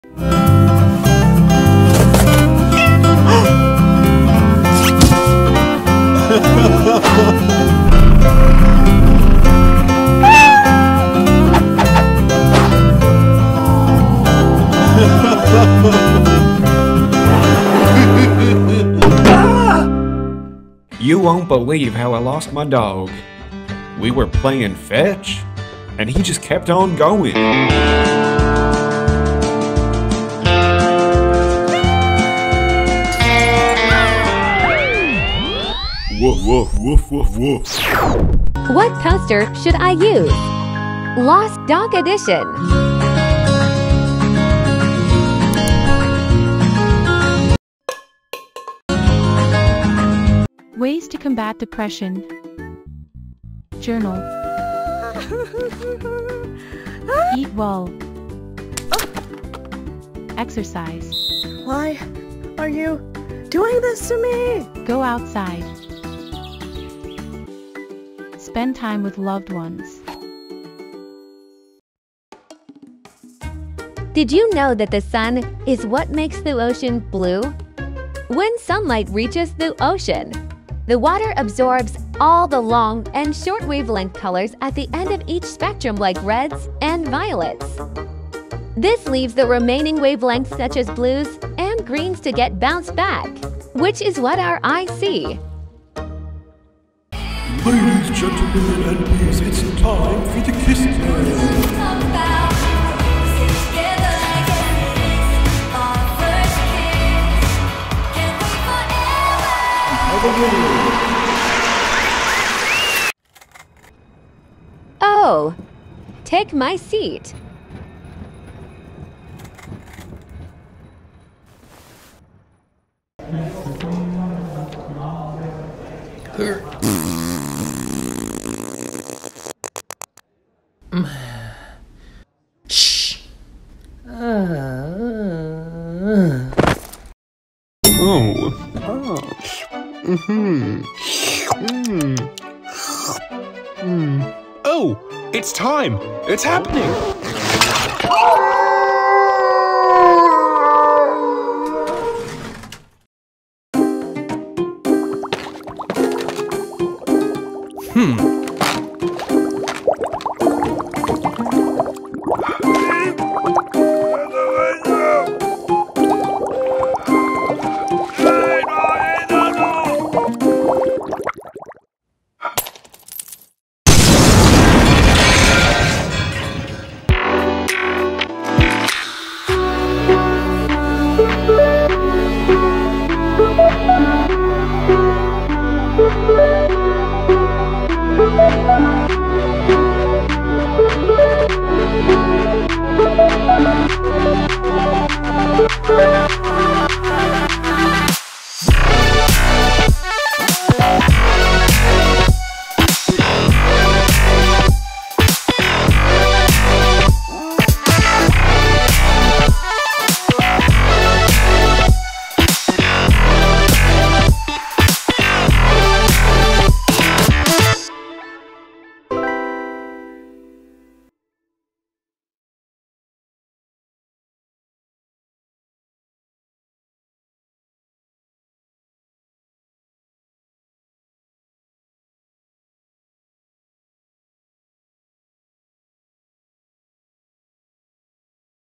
The BEST of Holler Shorts | Episode 12 | Cartoon Compilation | Hilarious Animated Cartoons
Cartoon Compilation by Holler. A roundup of this week's BEST cartoon episodes including Romeon, skunks, a wizard cat, and more!

animations, memes, 4k memes, viral, laughs, funny videos, fyp, foryou, best video, skunks, skunktember